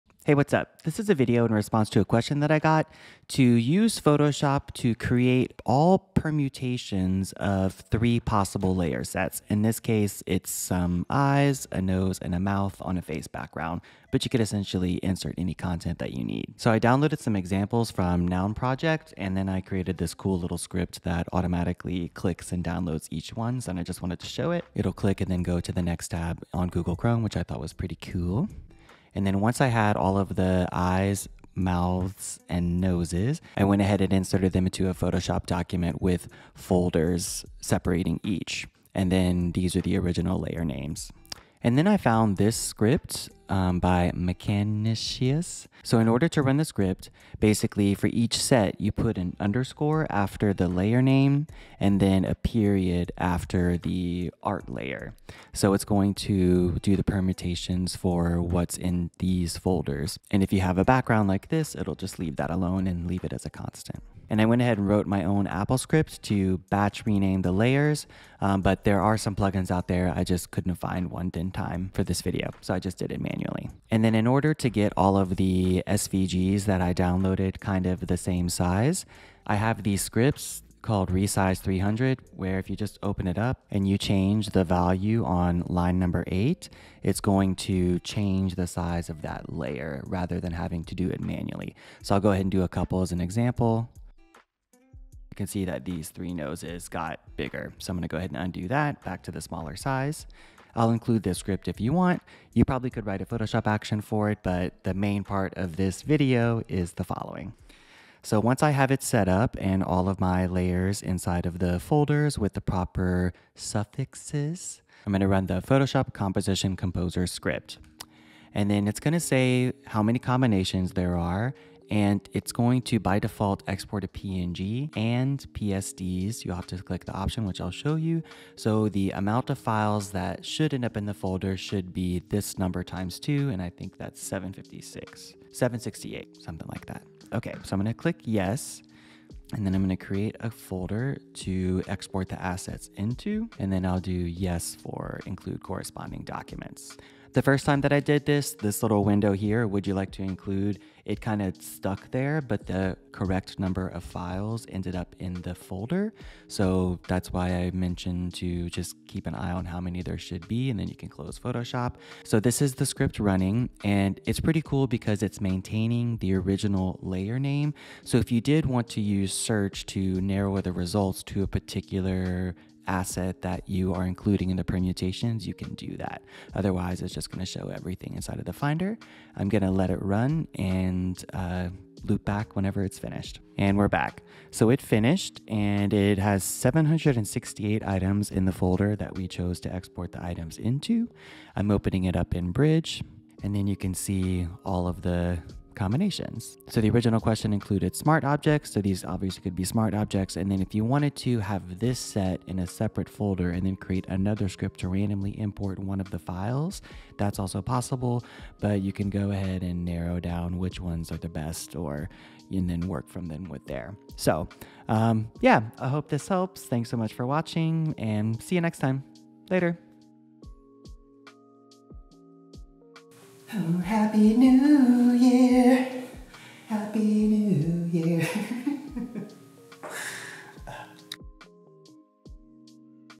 How to create all possible permutations in Photoshop
Here's a quick answer on how to create all possible combinations of 3 layer sets in Photoshop.